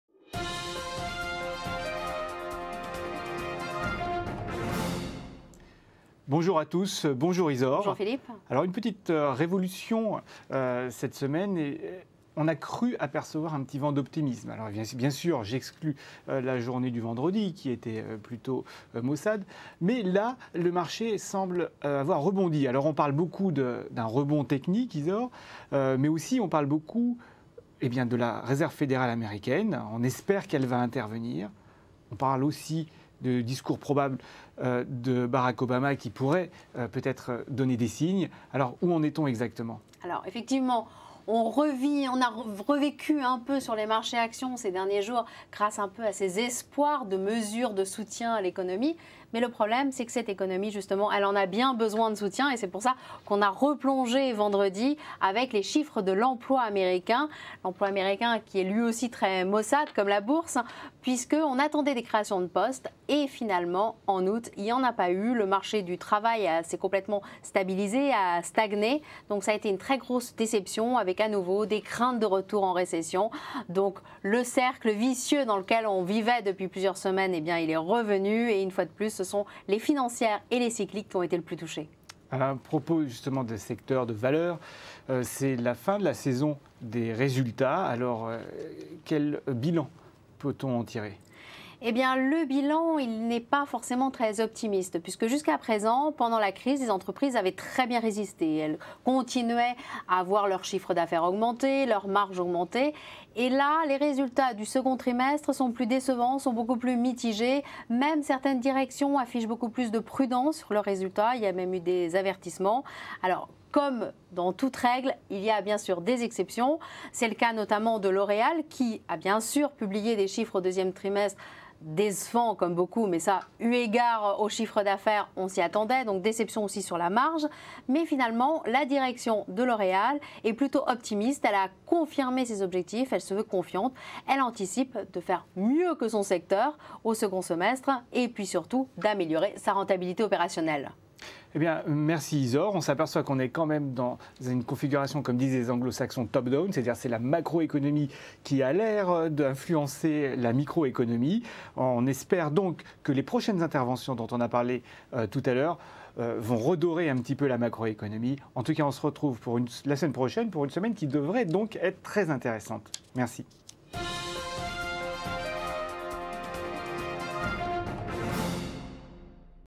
Espoirs de soutien de la Fed puis chute des bourses avec l'emploi américain
Après 4 séances d'un optimisme alimenté par l'espoir que la Fed prendrait rapidement des mesures pour relancer la croissance, la Bourse de Paris a fait l'objet de prises de bénéfices. Ces dernières ont été accentuées par la publication du rapport mensuel sur l'emploi américain. Alors que les économistes attendaient des créations de postes, le marché du travail est resté atone en août. Côté entreprises quelques déceptions sur les chiffres du deuxième trimestre, beaucoup de prudence mais L'Oréal, lui, reste confiant sur ses perspectives.